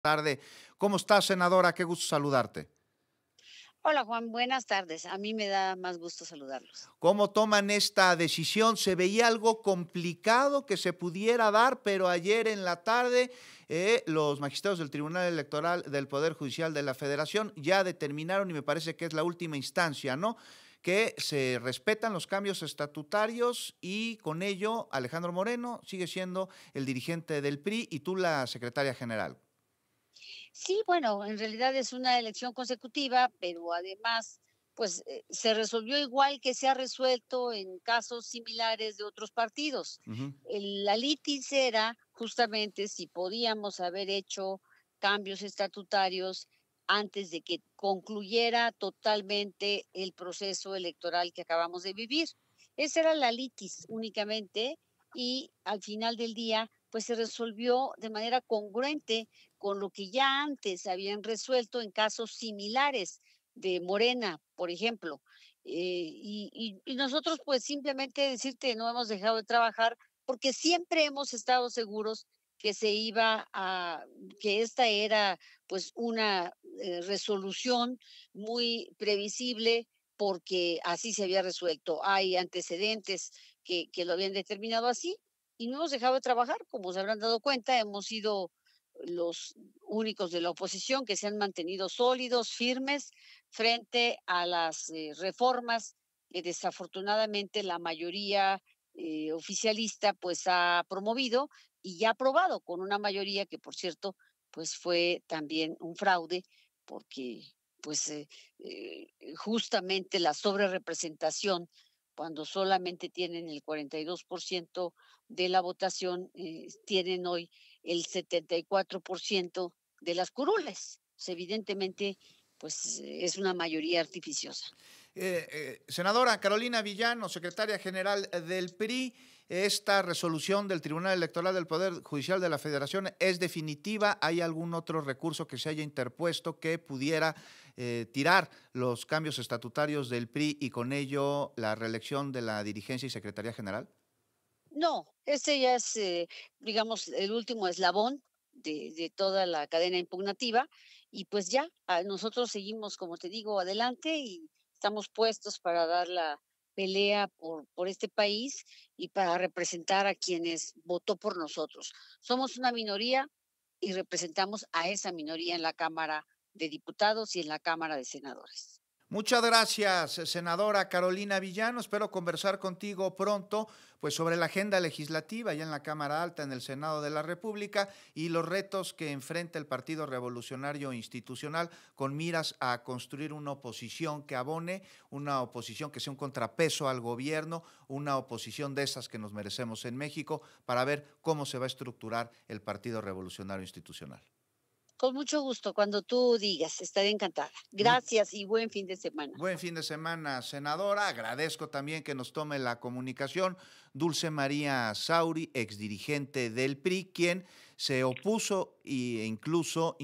Siempre estuvimos seguros de la resolución: Carolina Viggiano sobre reelección de ‘Alito’
Carolina Viggiano, senadora del PRI, se pronunció sobre la validación del TEPJF que ratifica a Alejandro Moreno como dirigente del Partido Revolucionario Institucional. Afirmó que el partido siempre estuvo seguro de esta resolución y destacó que, con esta decisión, continuarán siendo una oposición firme ante las reformas debatidas. “Estamos listos para dar pelea por el país”, afirmó.
Grupo Fórmula